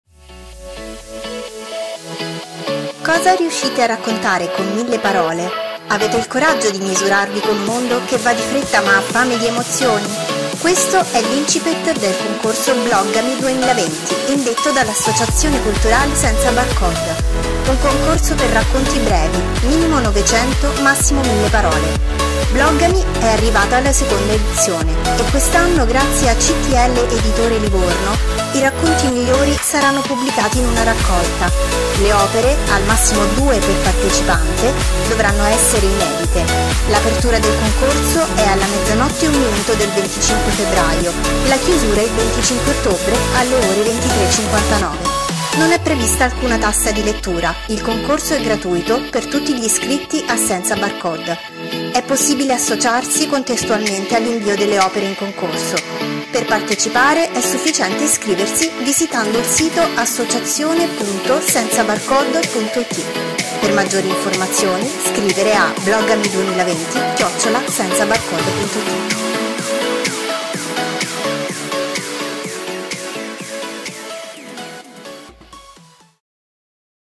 Bloggami 2020
Cosa riuscite a raccontare con 1000 parole? Avete il coraggio di misurarvi con un mondo che va di fretta, ma ha fame di emozioni? Questo è l'incipit del concorso bloggami 2020 indetto dall'Associazione culturale SenzaBarcode.
Un concorso per racconti brevi, minimo 900 e massimo 1000 parole. Bloggami è arrivato alla seconda edizione è quest'anno, grazie a CTL editore Livorno, i racconti migliori saranno pubblicati in una raccolta. Le opere, al massimo due per partecipante, dovranno essere inedite, l'apertura del concorso è alle 00.01 del 25 febbraio, la chiusura Il 25 ottobre alle 23:59. Non è prevista alcuna tassa di lettura, il concorso è gratuito per tutti gli iscritti a SenzaBarcode. È possibile associarsi contestualmente all'invio delle opere in concorso.